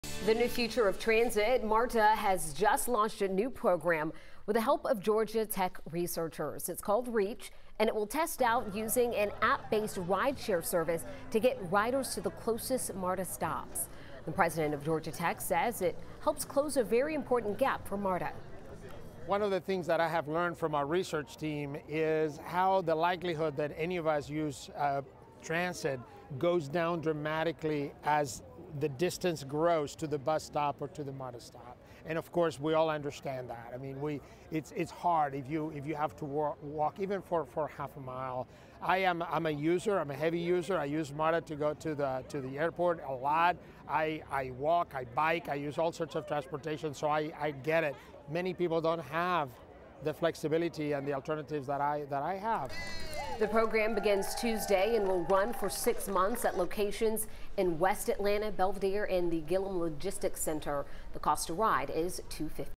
MARTA launches new program with the help of Georgia Tech researchers
It is called "Reach."